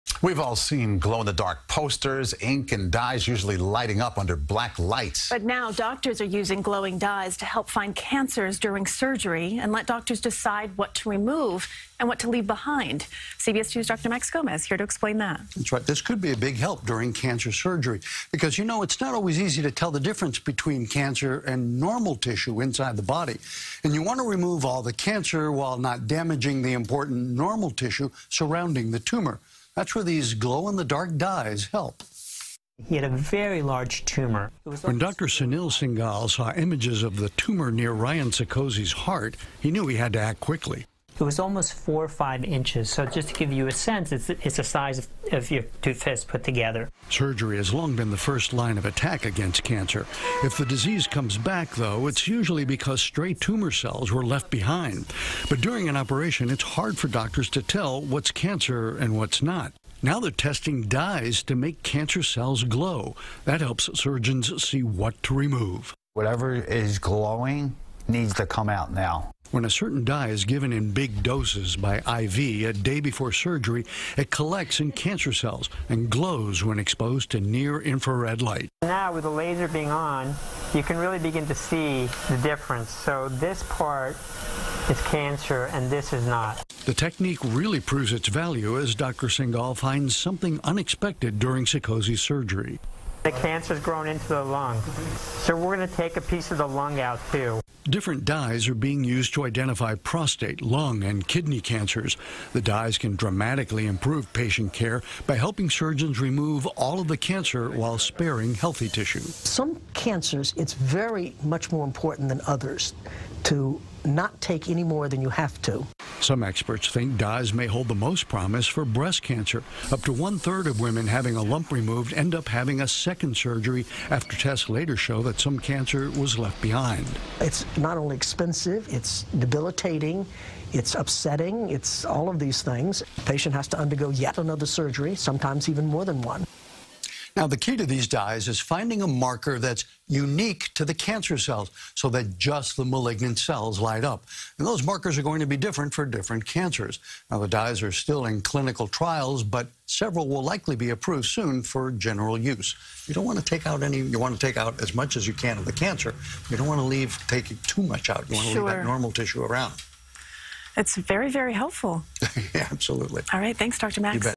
Doctors Using Glowing Dyes To Help Detect Cancers During Surgery
We've all seen glow in the dark posters, ink, and dyes usually lighting up under black light. Now, doctors are using glowing dyes to help find cancers during surgery and let doctors decide what to remove, and what to leave behind. CBS2's Dr. Max Gomez reports.